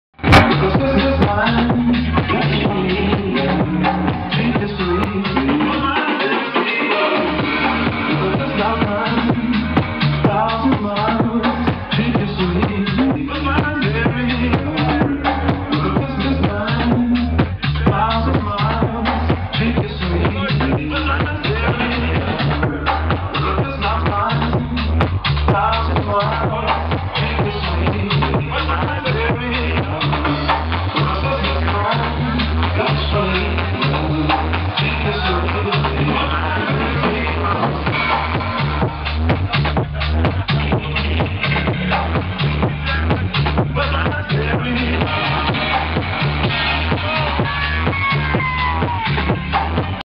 H20 Sunset Cruise with Dj Lady OH (2)
Party on the boat in Koh phangan with Dj Lady OH on January 24th, 2013 - Video 2